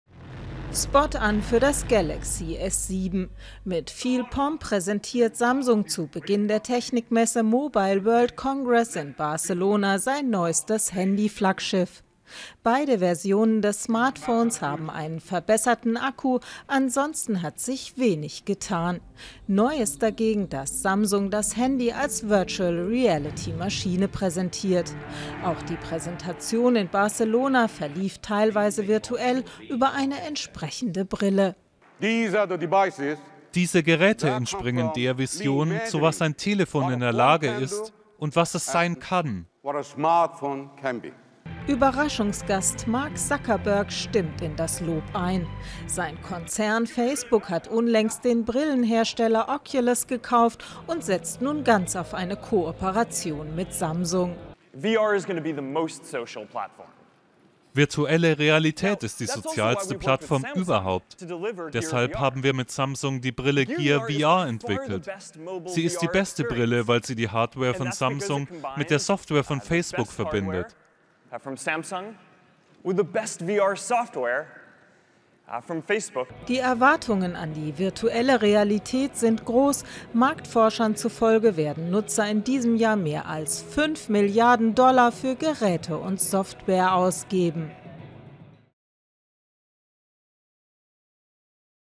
Mobile World Congress: Zuckerberg preist virtuelle Realität
Zum Auftakt der weltgrößten Mobilfunkmesse Mobile World Congress in Barcelona wagt Facebook-Chef Mark Zuckerberg einen Blick in die Zukunft. Demnach werden Virtual-Reality-Geräte immer wichtiger. Deshalb arbeitet Facebook bei der Entwicklung mit Samsung zusammen.